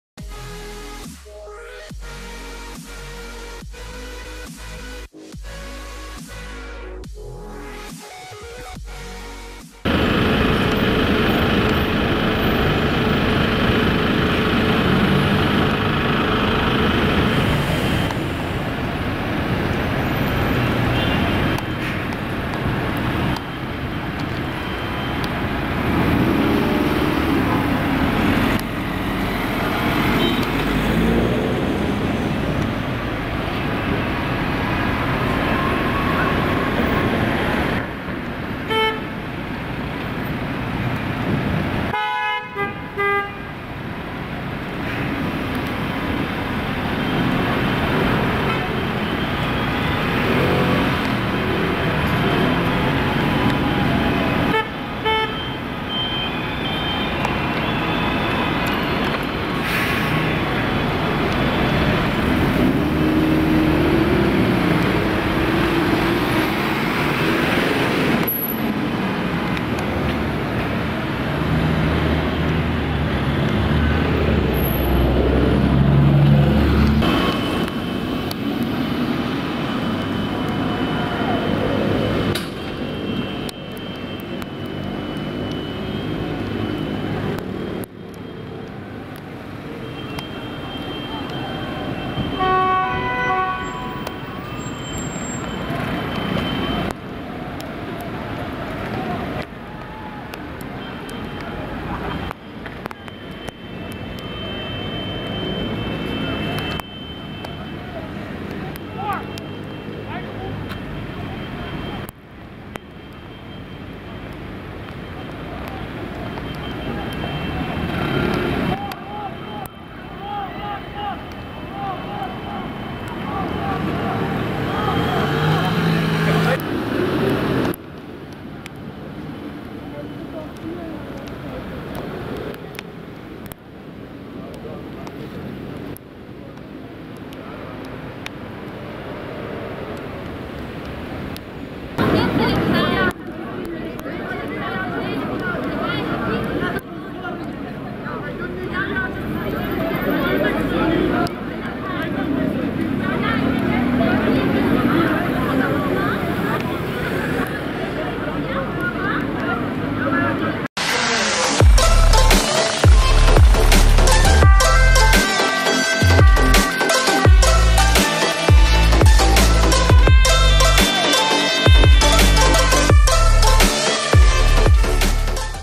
[Knalt op stoeprand] Mobiele Eenheid ingezet voor Pro-Palestina-demonstratie in Den Haag!
U kijkt naar: [Knalt op stoeprand] Mobiele Eenheid ingezet voor Pro Palestina demonstratie in Den Haag!

Info: N.V.T
Datum: 12 Mei 2021

Door: Stefan

Camera: Panasonic HCV-720

Copyrights: Deze video is niet te gebruiken zonder toestemming.